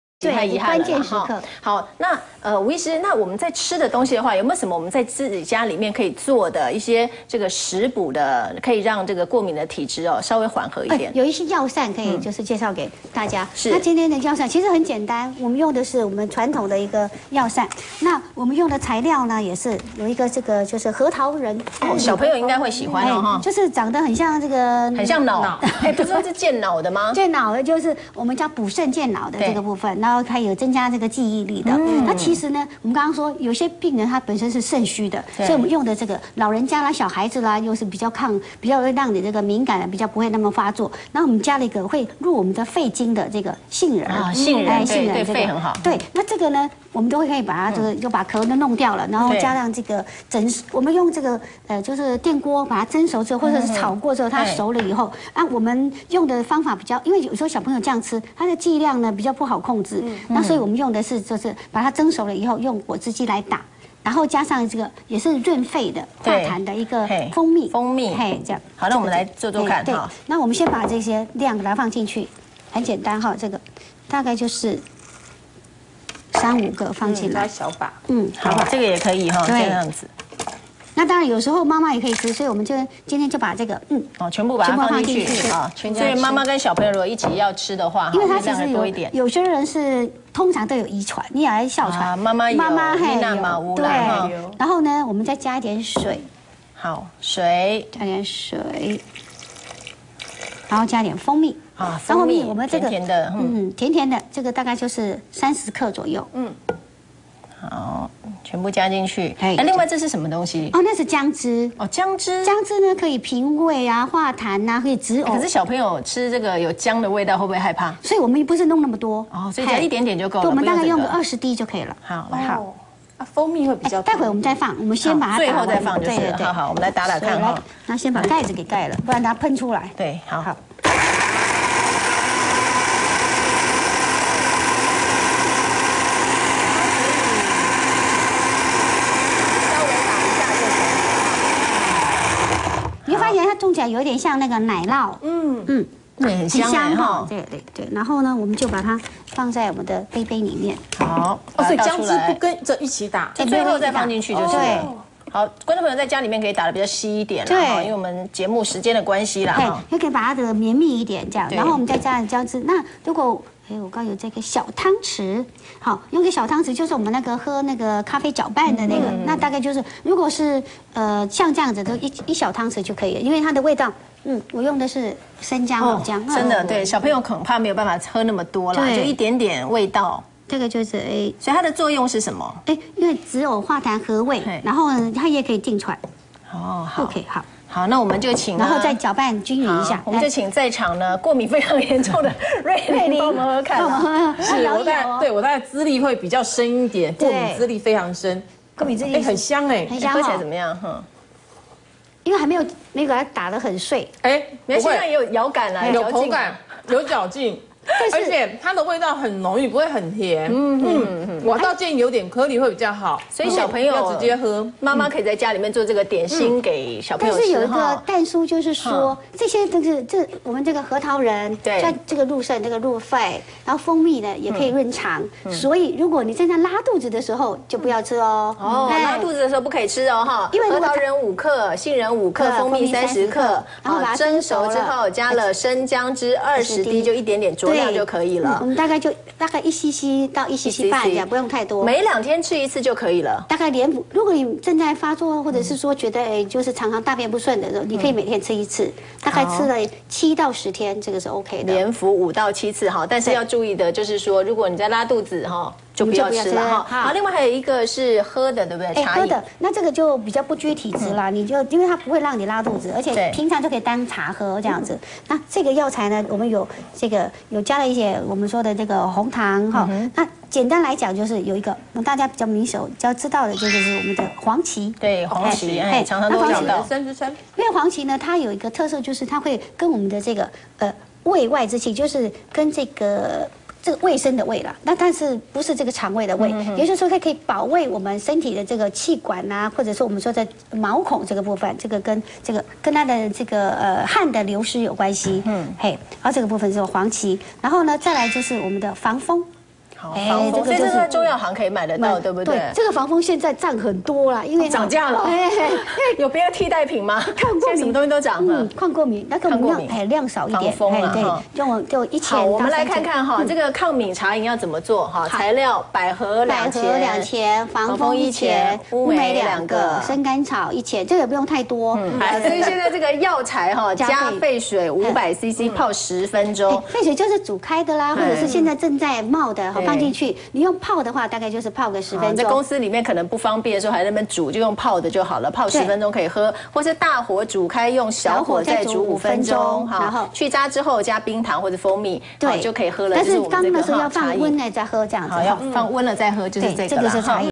核桃仁+杏仁+蜂蜜抗過敏點心 增強免疫力 | 養生我知道 | 三立財經台
【抗敏點心】
核桃仁5克，杏仁5克，蜂蜜30克，三者混合後蒸熟加生薑汁20滴，一次服完。每隔兩日服上述藥方一次，連服5～7次。
【抗敏茶飲】
百合2錢、防風1錢、烏梅2個、生甘草1錢、紅棗2枚、柴胡1錢、五味子1錢、冰糖或蜂蜜適量
【做法】
將以上藥材加沸水500CC，浸泡10分鐘或用大火煮開後用小火再煮5分鐘，去渣後，稍涼後加入冰糖或蜂蜜攪拌均勻即可當茶喝。